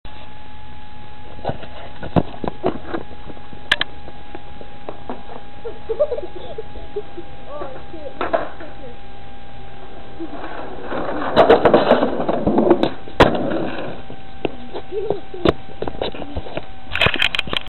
hespeler skater boys
Look at this ollie over someone